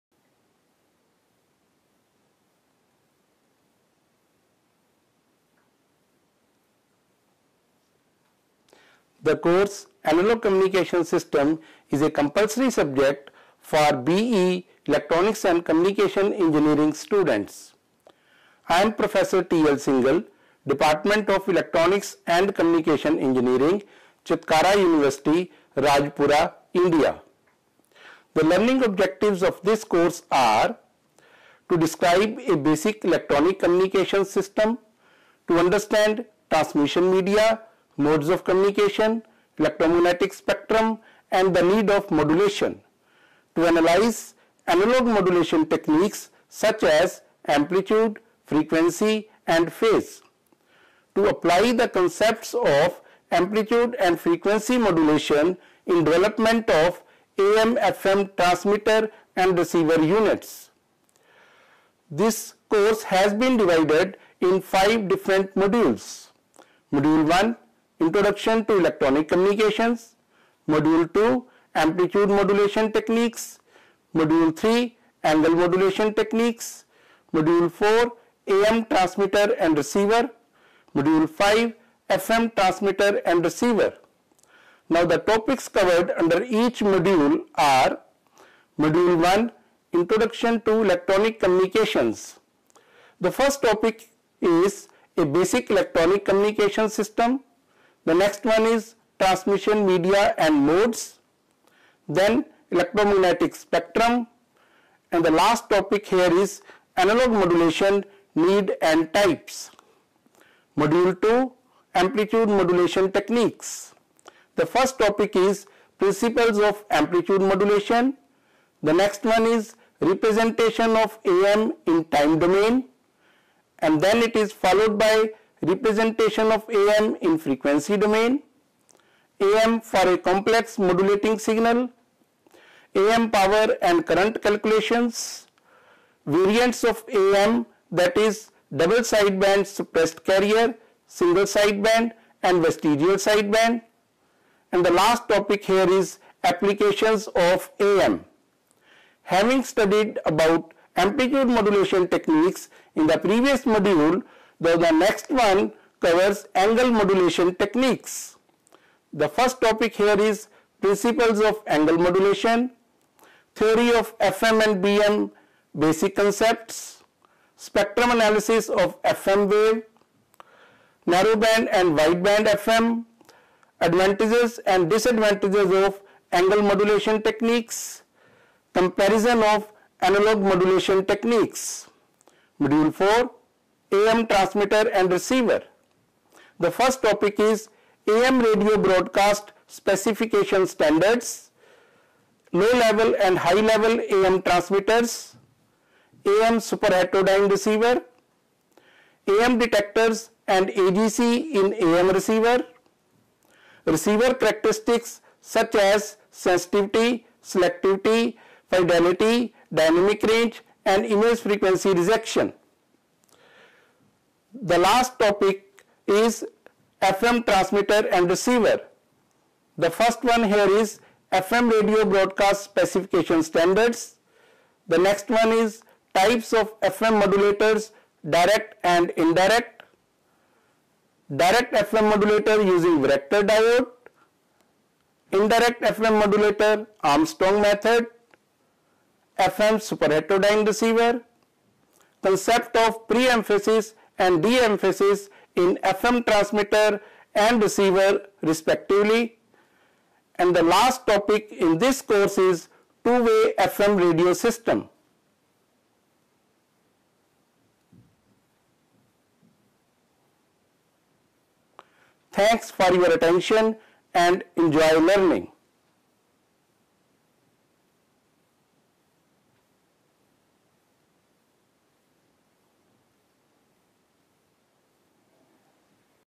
Introduction to Analog Communications Course by Prof. T.L. Singal
This video presents a brief overview of major contents of Analog Communications course.